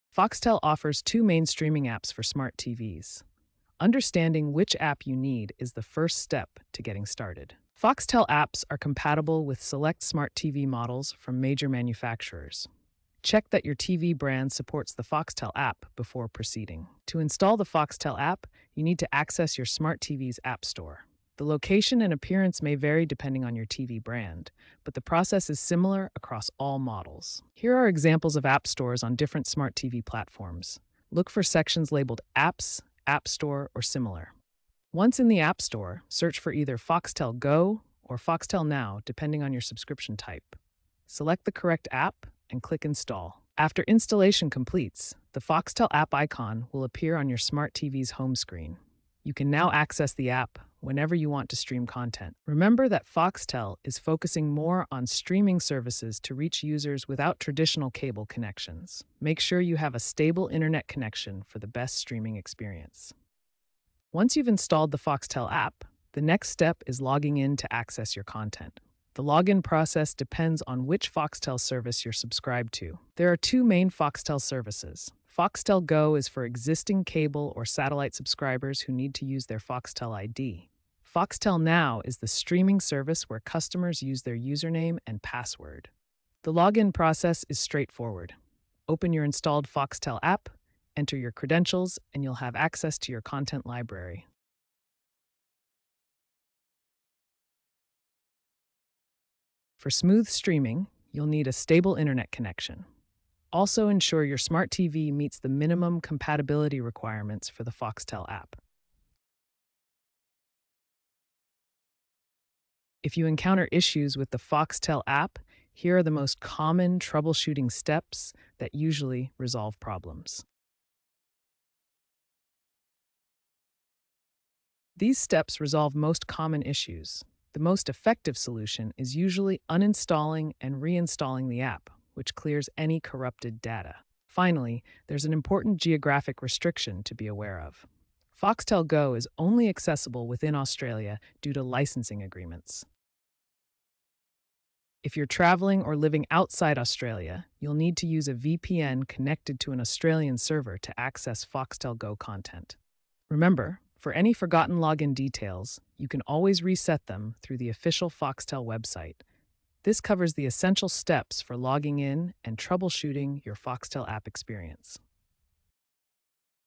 How To Download Foxtel App On Smart TV – Install & Login Tutorial
Unlock a world of entertainment with the Foxtel App for your Smart TV. Our comprehensive installation and login tutorial guides you through the simple steps to download and set up the app seamlessly. Enjoy access to a vast library of movies, shows, and live sports, all from the comfort of your living room. Elevate your viewing experience today and immerse yourself in premium content with just a few clicks.